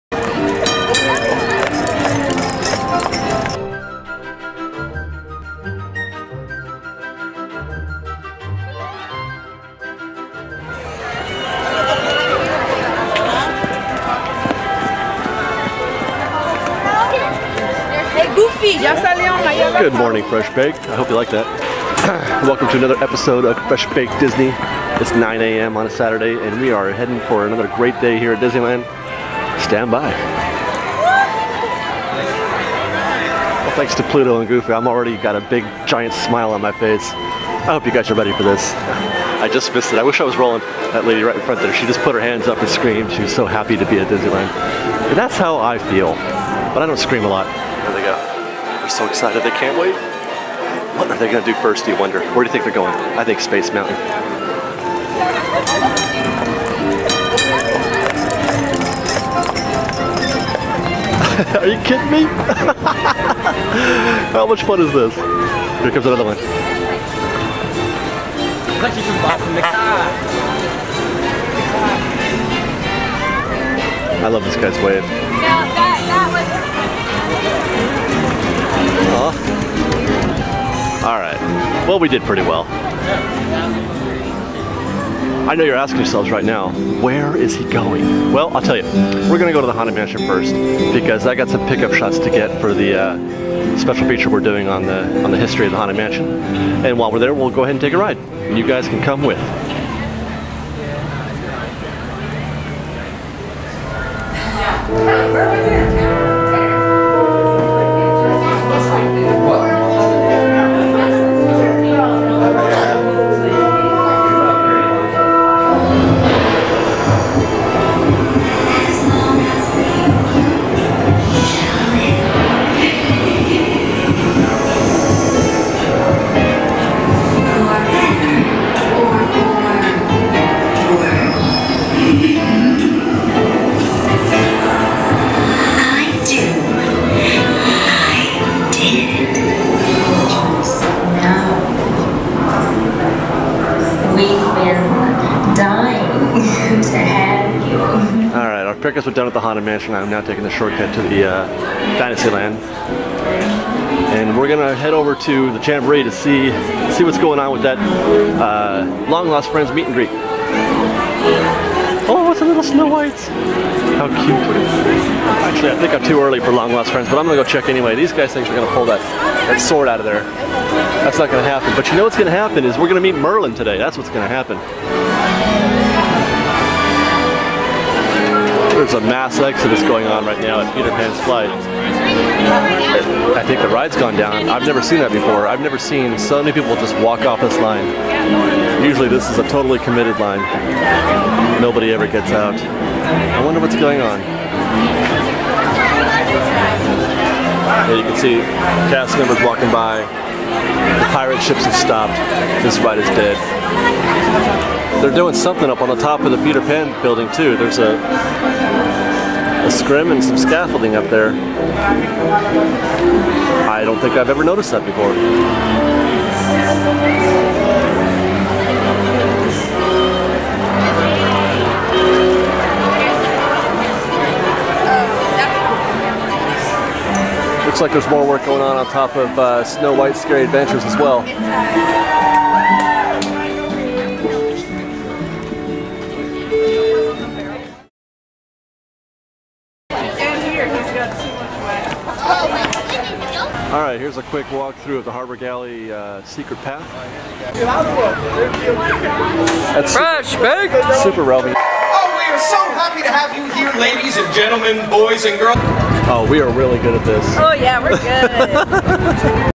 Disneyland Update 8.24.13 Pt. 1 [HD]
And we're back doing what we do best.  Going to the park and having a great time.  We're en route to the Long Lost Friends meet, but make a few stops on the way.

This video was shot in HD on August 24th, 2013 at Disneyland in Anaheim, California.

for detailed trip reports, ride throughs, special features and more.  Youtube is just the beginning!